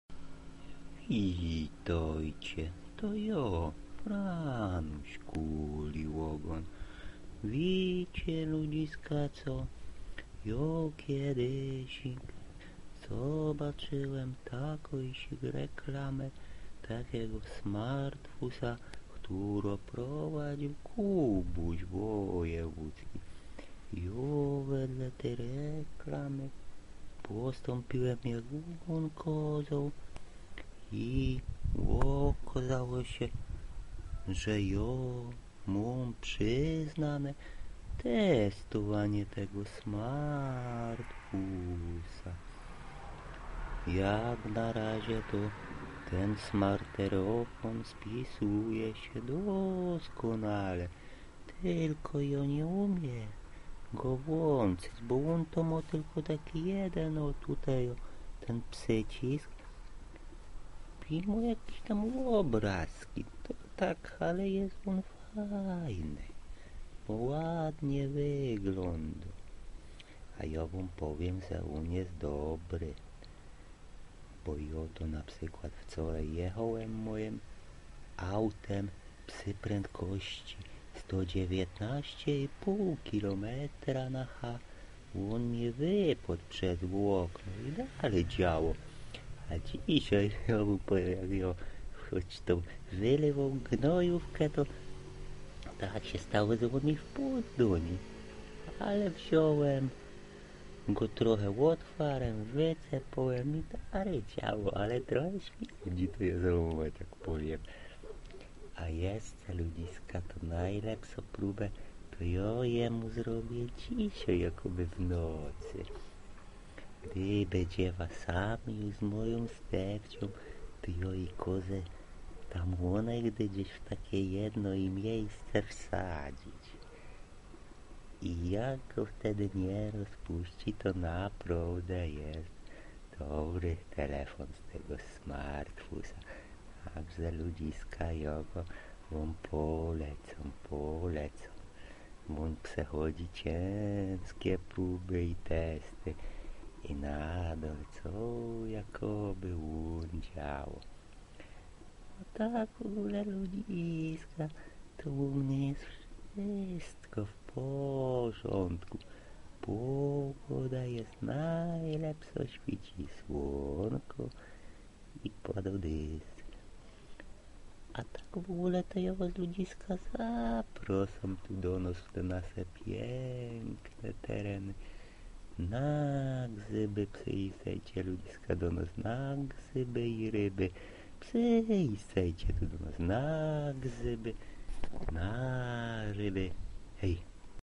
Test Smartfona udostepnionego przez PLAY
Oj ten smartyfon przechodzi okropnie trudne testy.